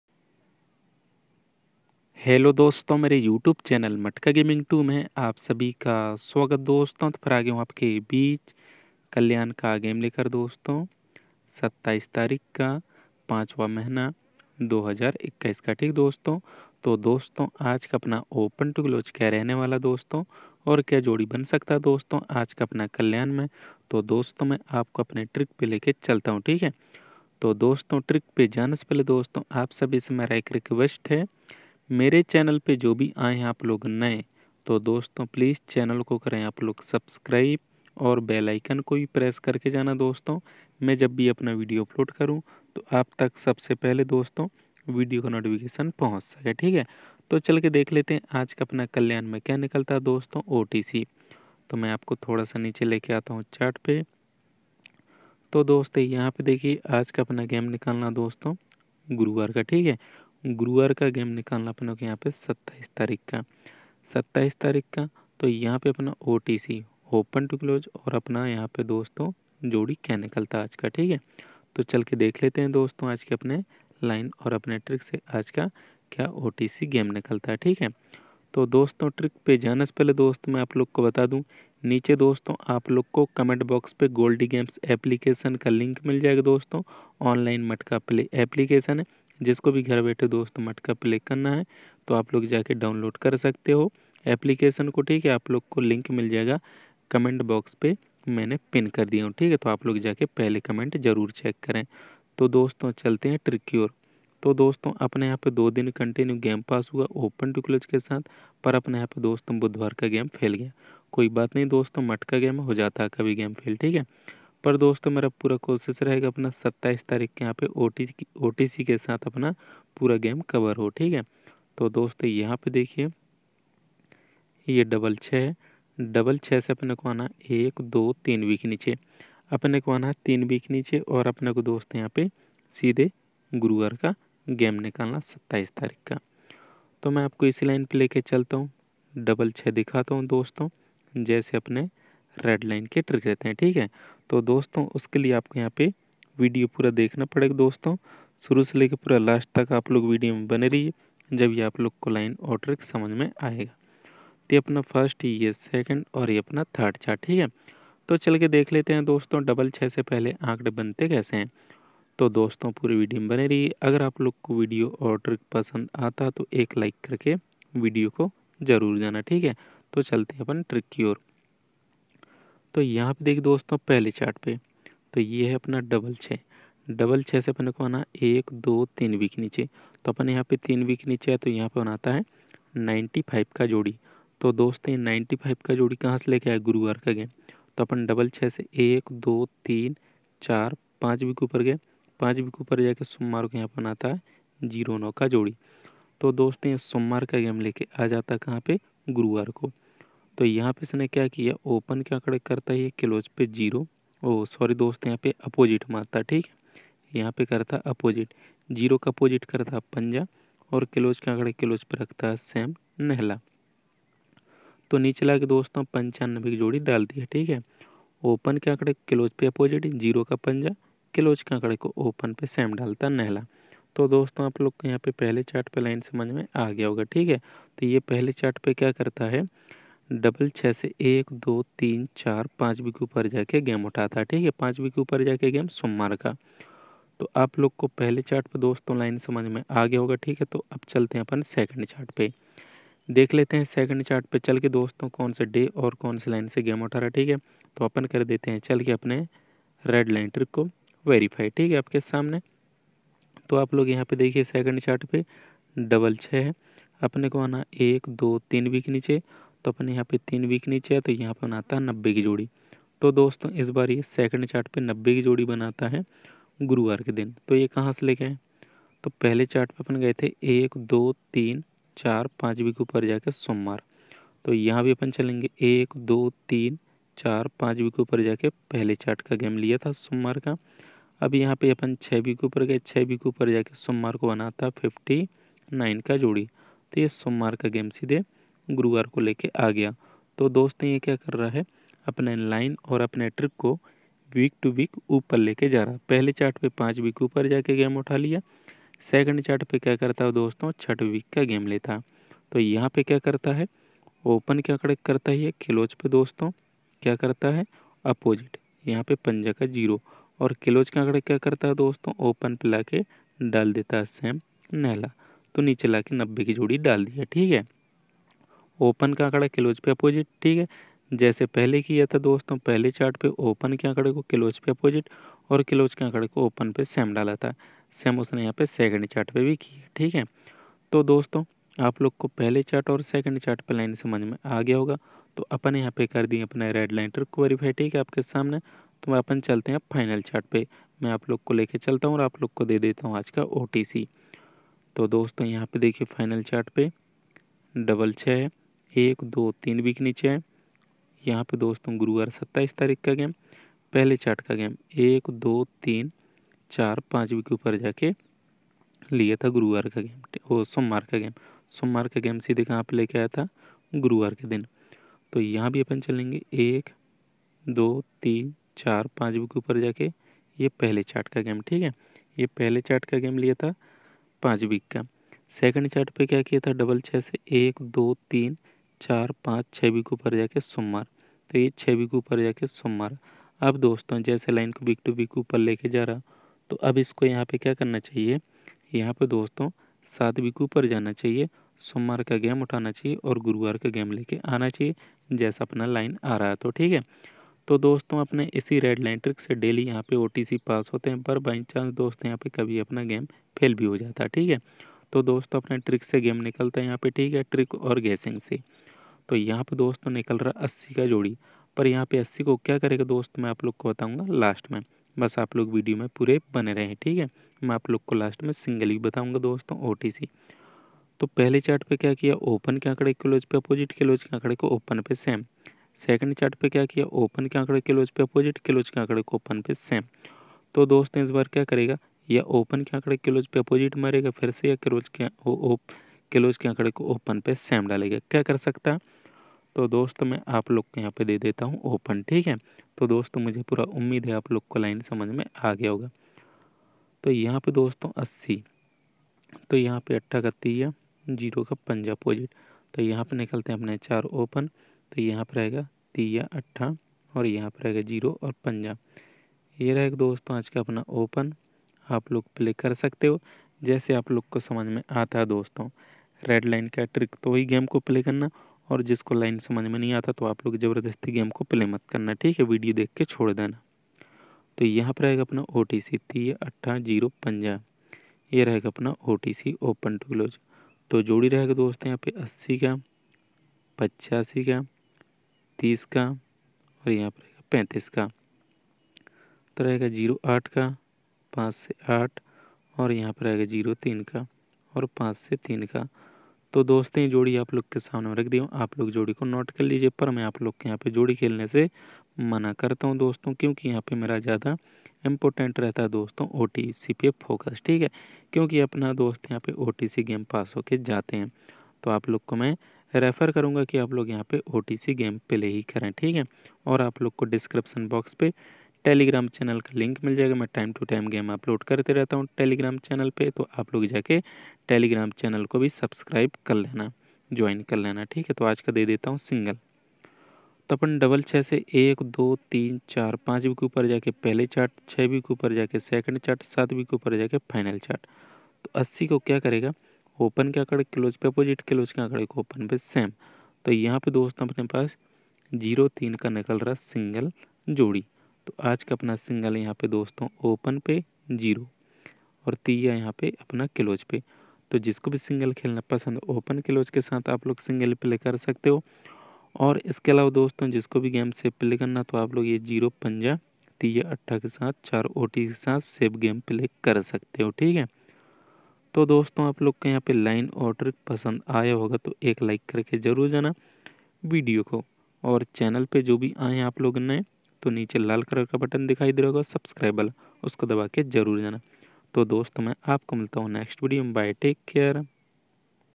KALYAN MATKA 27-05-2021 | single Jodi | open to close | red line trick | कल्याण | today Kalyan satta
HELLO DOSTON

TODAY KALYAN MATKA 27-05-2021 single Jodi open to close red line trick Kalyan satta matka

Namaskar dosto aap sabhi ka swagat hai aaj ka Kalyan matka game me, aaj me aap sabhi ko mere red trick se bahut hi jabardasth line or guessing ke sath Kalyan ka game de raha hu agar aap logo ko Mera trick pasnd Aaya ho to CHANNEL ko SUBSCRIBE kare VIDEO ko LIKE or SHARE jarur kijiye

Kalyan today 27/05/2021 single Jodi.